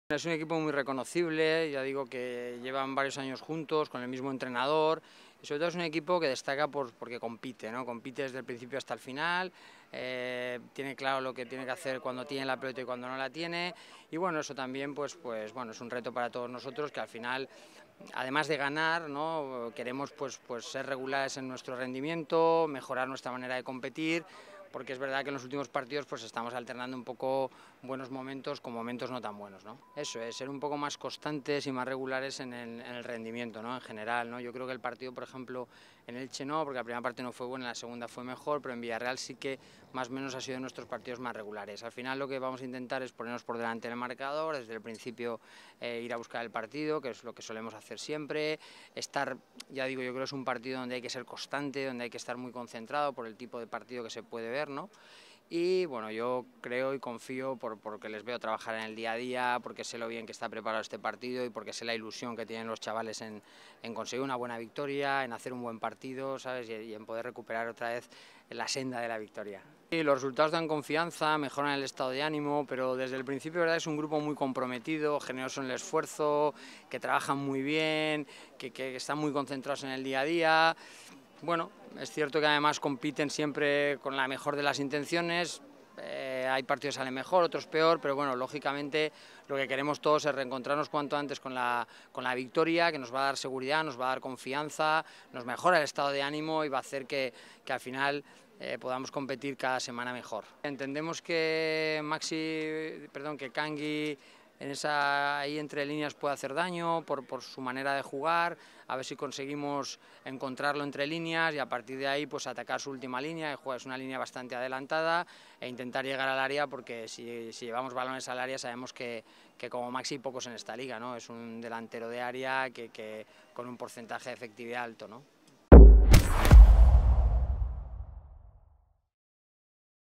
🗣️ DECLARACIONES DE CHEMA SANZ EN LA PREVIA DEL VALENCIAGETAFE🦇🟣
Chema Sanz, asistente del cuerpo técnico del Valencia CF, atiende a VCF Media antes del encuentro ante el Getafe CF.

 Chema Sanz, makes a preliminary assessment of the match against Getafe CF.